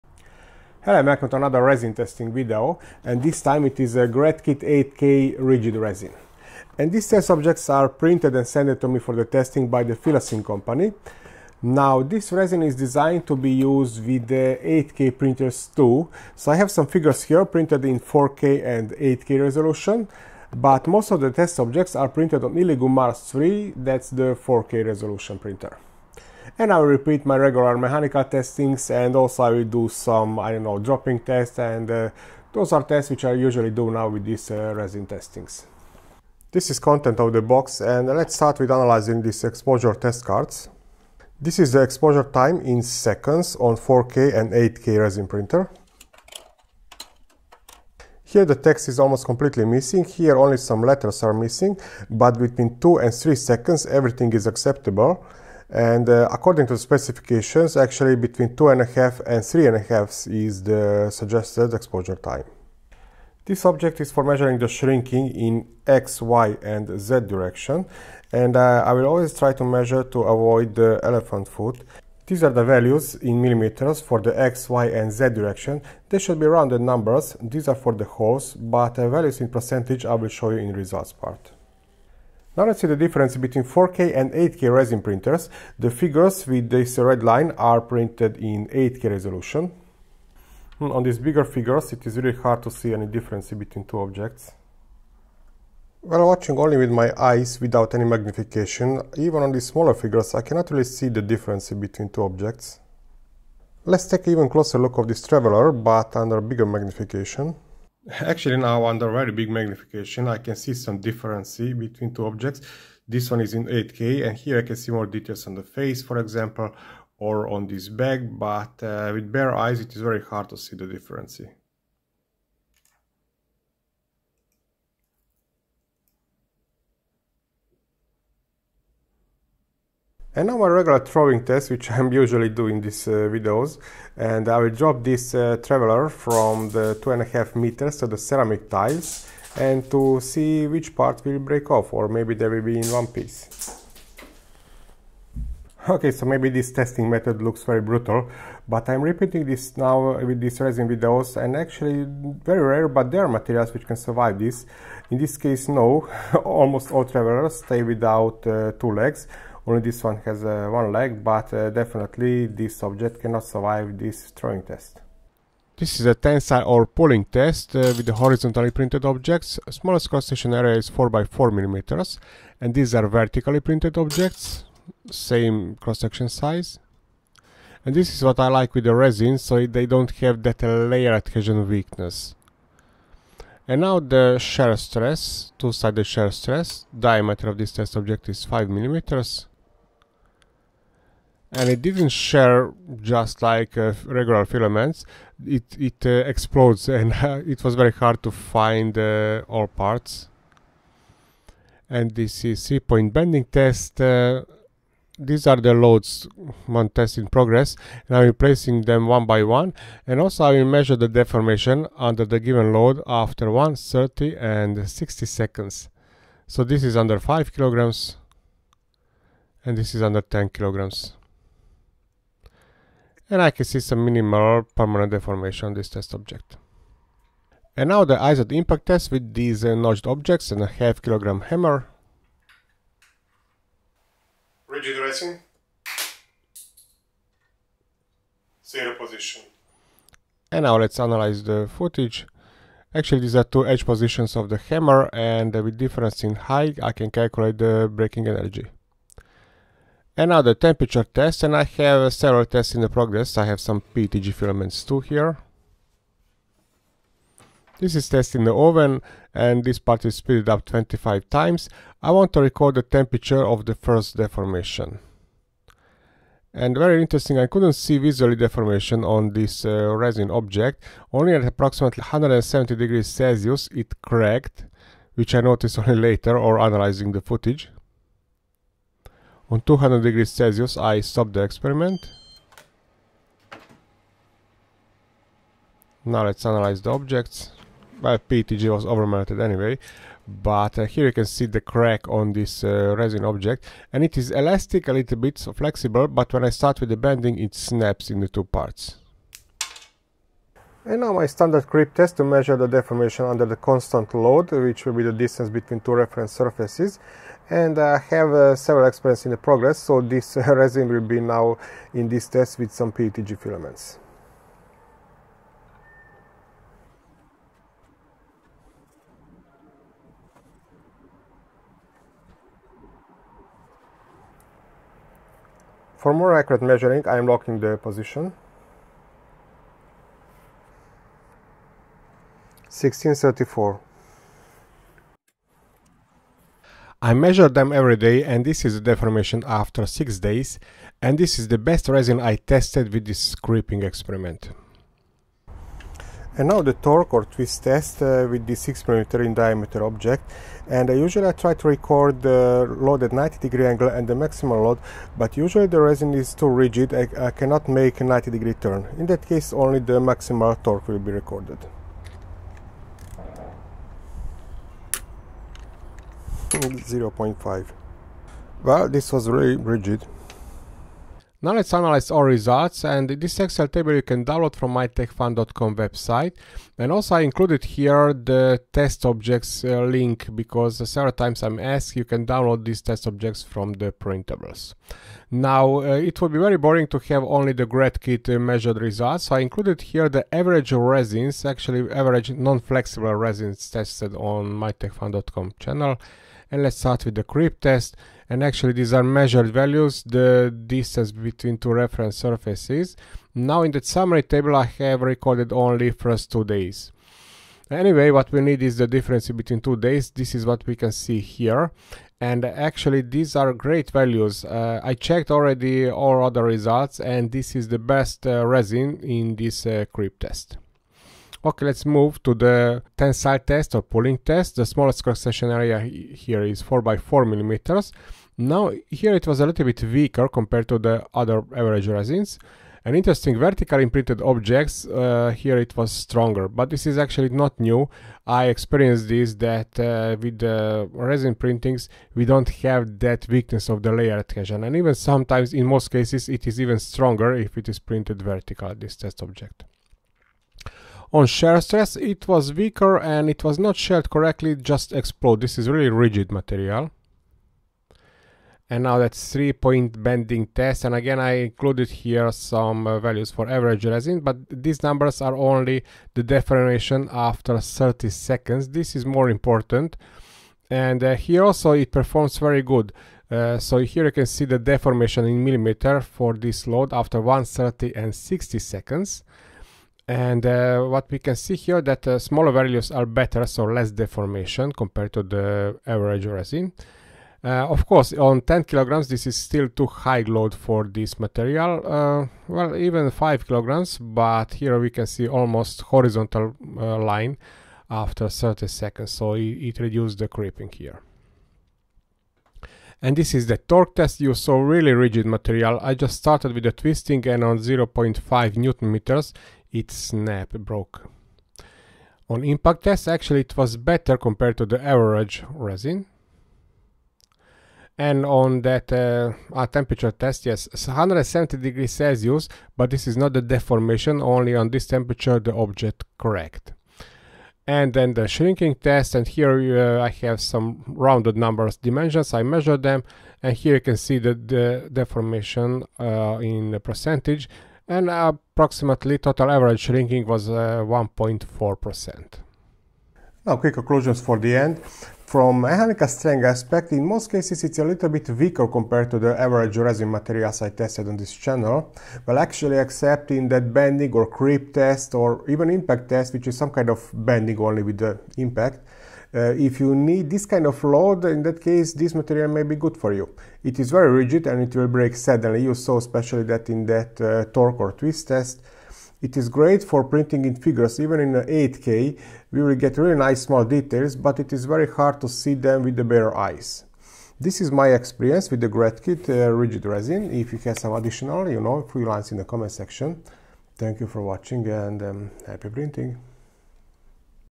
Gratkit 8k Rigid Resin review
GratKit 8K Rigid 3D printing resin is designed to be compatible with 8K high resolution printers to print high precision models. Factors of low viscosity and good fluidity helps the liquid resin for speedy printing and curing. In this video mostly the mechanical strength is tested.

Contents:
0:00 Introduction
0:36 Exposure test cards
1:03 Shrinking
1:24 4k vs 8k figures
2:12 Dropping test
2:45 Tensile (pulling) test
2:55 Layer adhesion
3:04 Shear test
3:22 Bending test
3:48 Impact test
4:10 Temperature test
5:02 Creep test
5:46 Torque test
6:12 Results
9:04 Conclusions